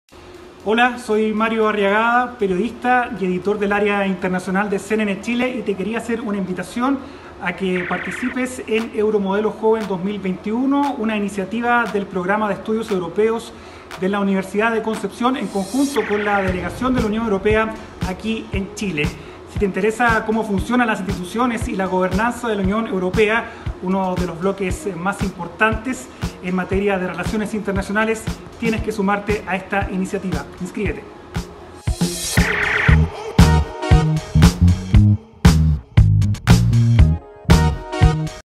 Saludos de Mario Arriagada/ Euro Modelo Joven 2021
Mario Arriagada, periodista del área internacional de CNN Chile te invita a participar del nuevo Euro Modelo Chile 2021. Organizado por UE en Chile y nuestro Programa de Estudios Europeos con apoyo de Fundación Revel y Nuevas Generaciones.

¿Te interesa?